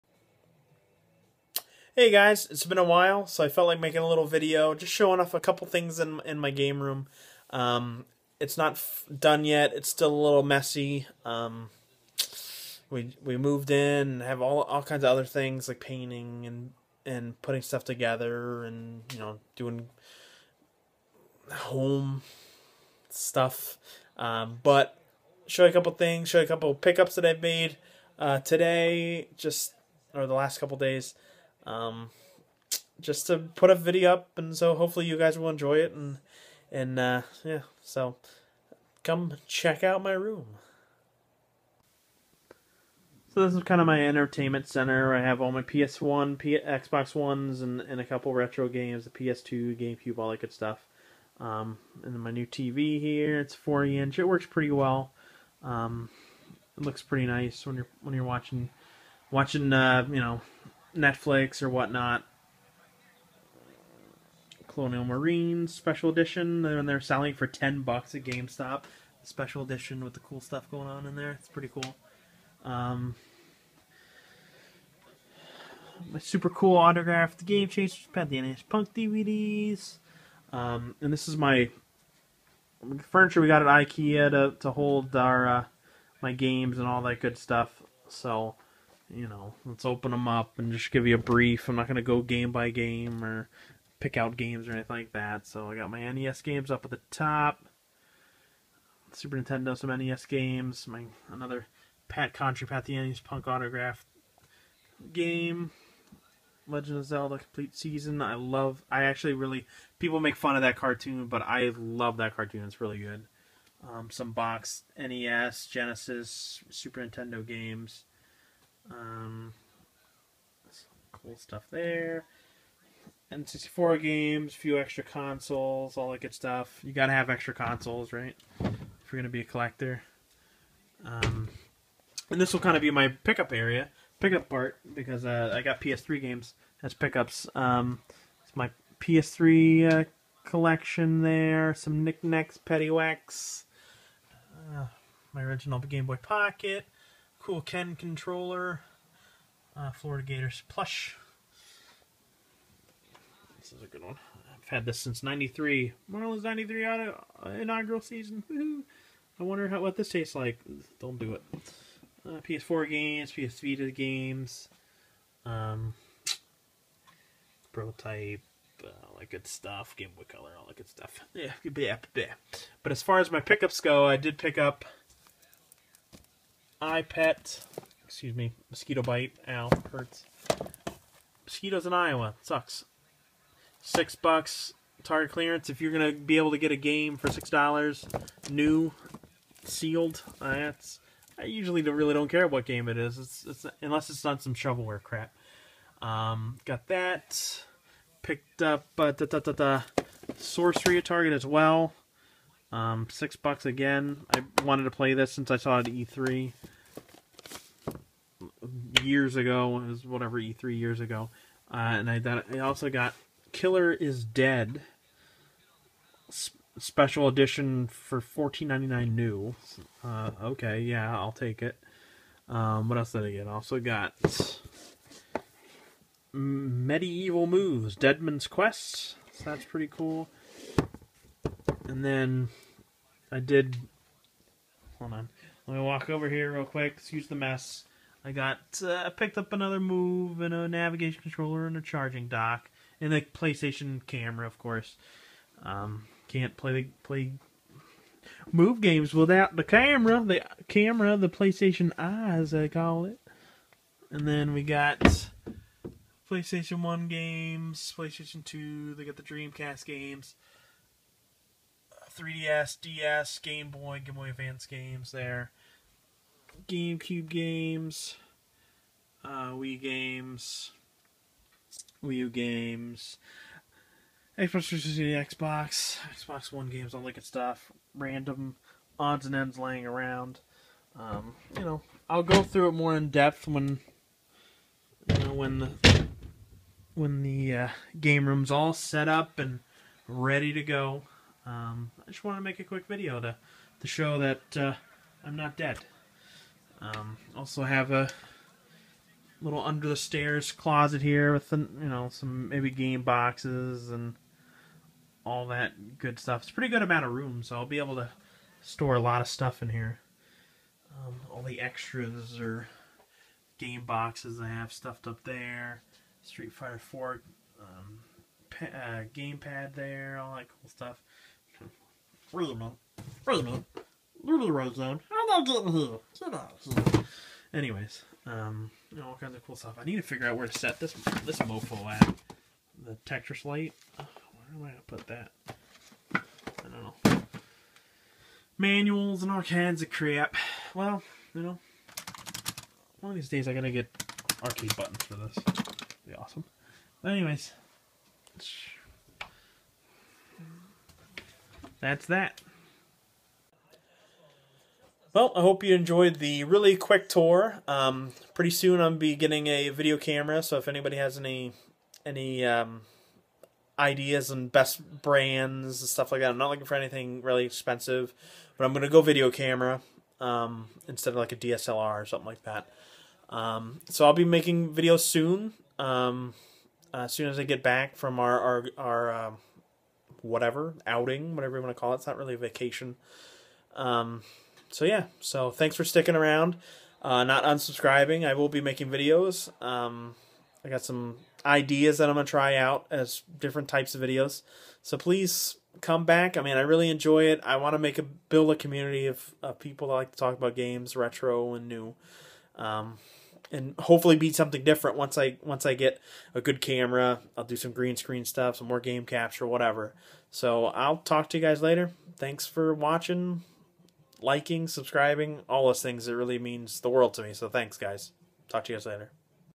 Game Room Update
Doing a good old game room tour. It's not quite finished and still kinda a mess but figured I'd give a peek.